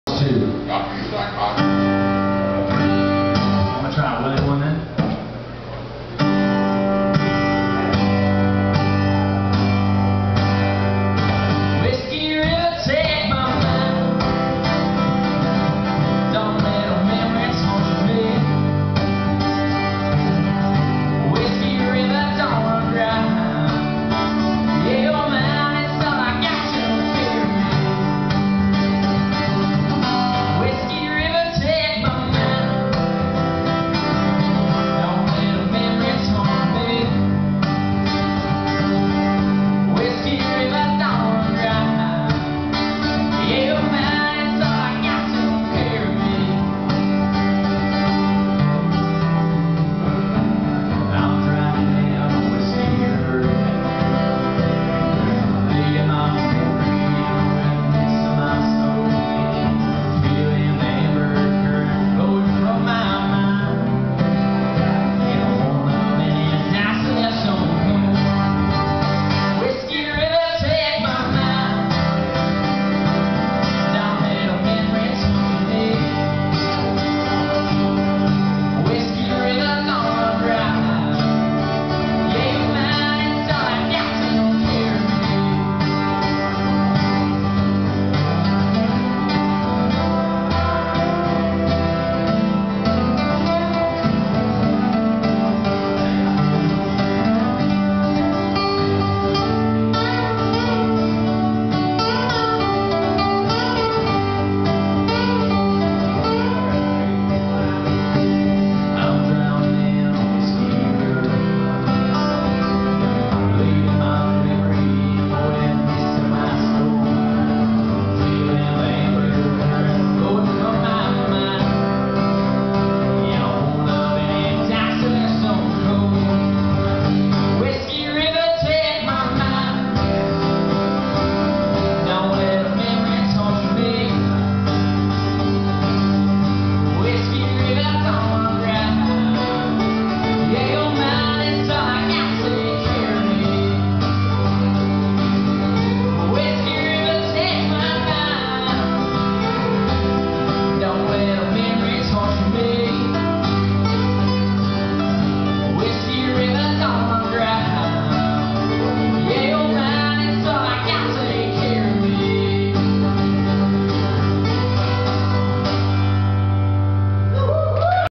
Jonathan Fox
Live @ Flithy McNastys in the Fort Worth Stockyards during the TOSSM RADIO Acoustic Series. 6/1/08